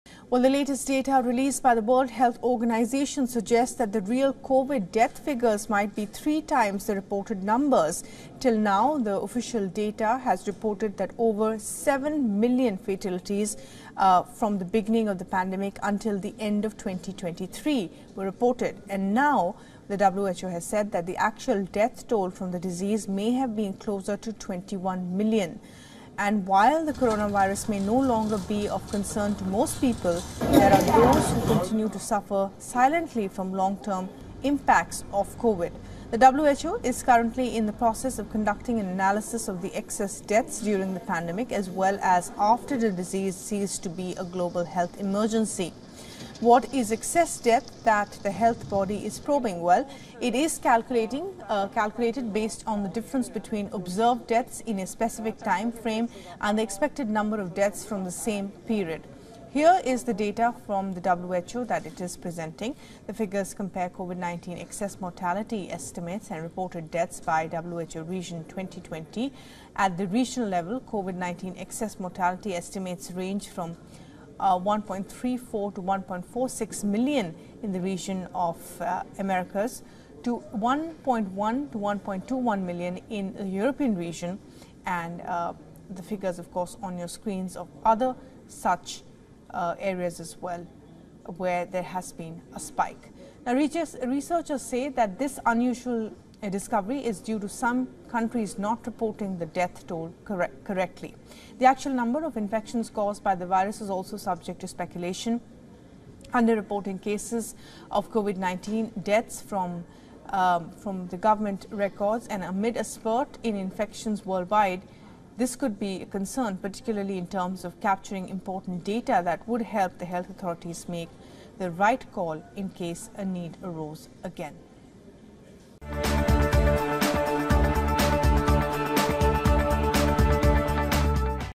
Covid19: Global death toll may be triple the reported deaths; countries didn't report toll correctly
The latest data released by the World Health Organisation suggests that the real Covid death figures might be three times the reported number. Till now, the official data has reported over 7 million fatalities from the beginning of the pandemic.